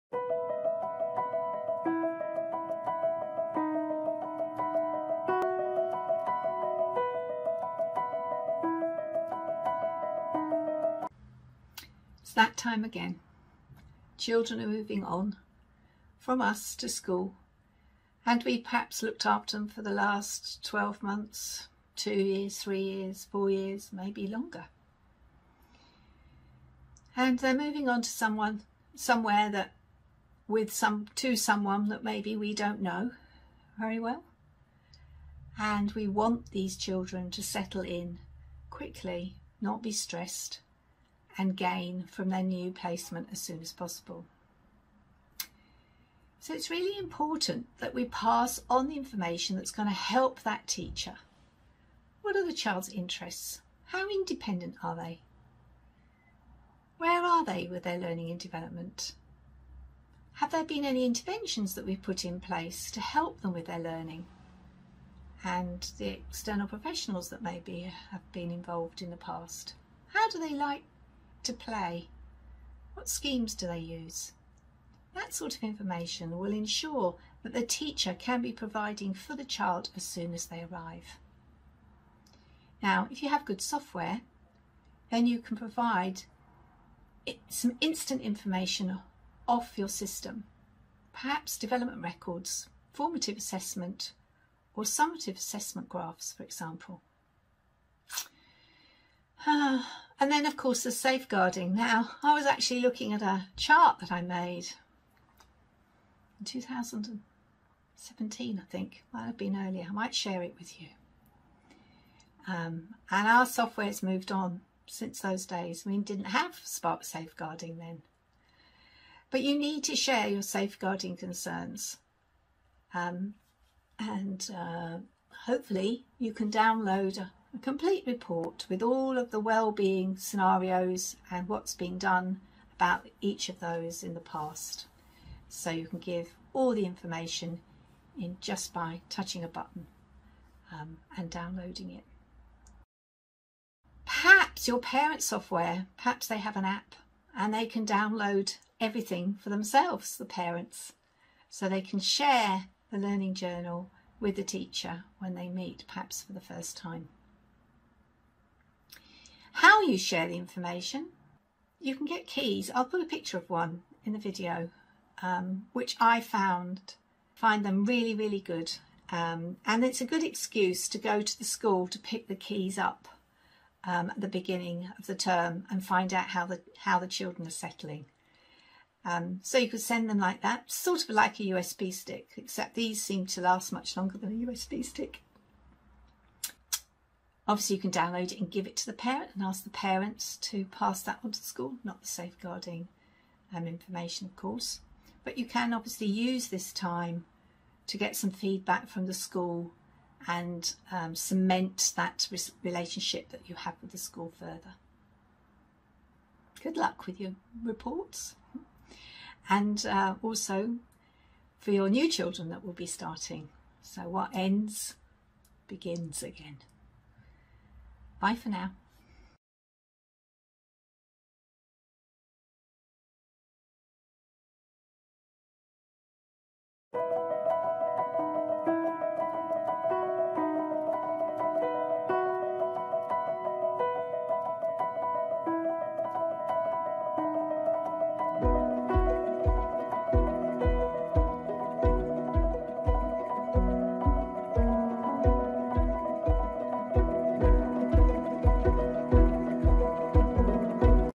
Outstanding EYFS - Transition Information
Aspiring for practice that is outstanding EYFS now is the time to compile the transition information as children leave for school.

This needn’t be a headache!  So long as systems are in place to make the process quick and easy.  Interestingly, I have seen Facebook posts where day nurseries and preschools are not bothering or being very relaxed about information sharing.

We accept that the early years is the most important stage to build brain connections, but we don’t equip the next educator with information to ensure the child’s needs are met from day one?  Doesn’t make sense to me!

Do share your ‘best practice’ with transitions.

Key Moments
0.20 - Moving onto school
0.35 - Settle in quickly
0.43 - Information to help the teacher on arrival
1.23 - Instant information with software
1.40 - Safeguarding
2.24 - Utilising Parent App
2.37 - USB Key
2.50 - Linking with school
3.05 - Parents sharing with school